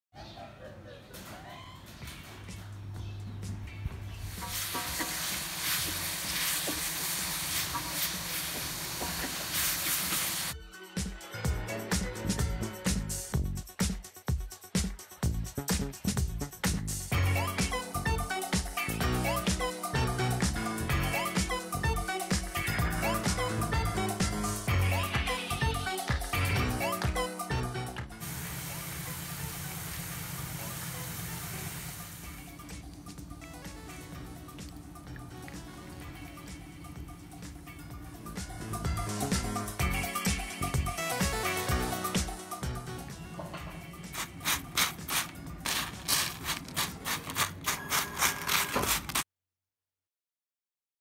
Blissful Kitchen teaches Fried Garlic
Learn how we make Fried Garlic garnishing at our Bed & Breakfast in Nyaung Shwe, Myanmar.